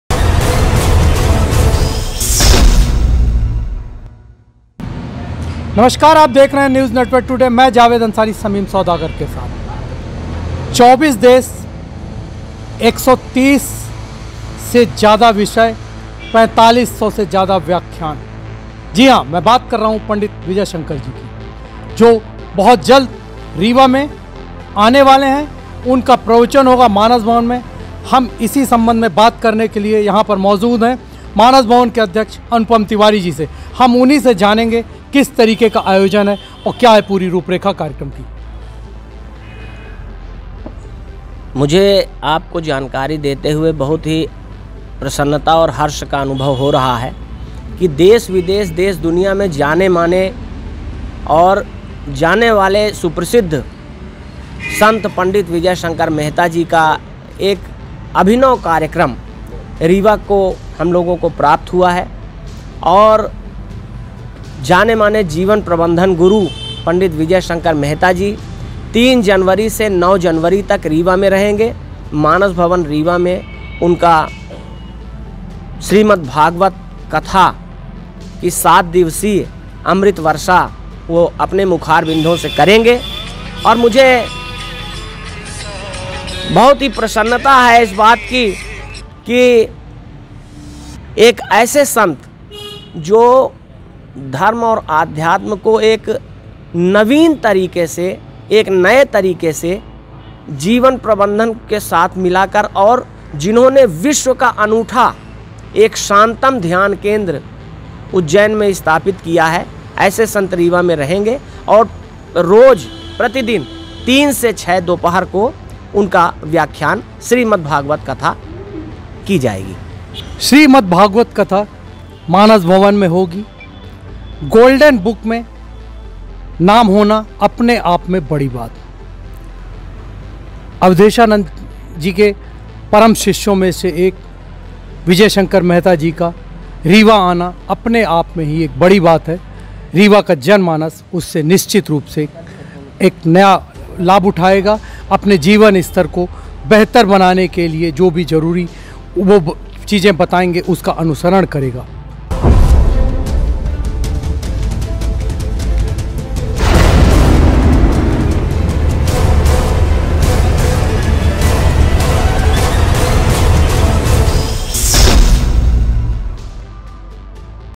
गोल्डन बुक विनर विजय शंकर मेहता नए साल में रीवा में ||NNT
गोल्डन बुक विनर विजय_शंकर_मेहता नए साल में रीवा में

ज्ञान की गंगा बहेगी मानस भवन में अवधेशानंद जी के परम शिष्य विजय शंकर मेहता जी जिनके नाम सैकड़ों ऐसे रिकॉर्ड हैं जिसके बारे में सोच पाना तमाम लोगों के लिए संभव नहीं है नए साल में रीवा के मानस भवन में एक हफ्ते तक भक्ति भाव में सरोवर करेंगे रीवा के लोगों को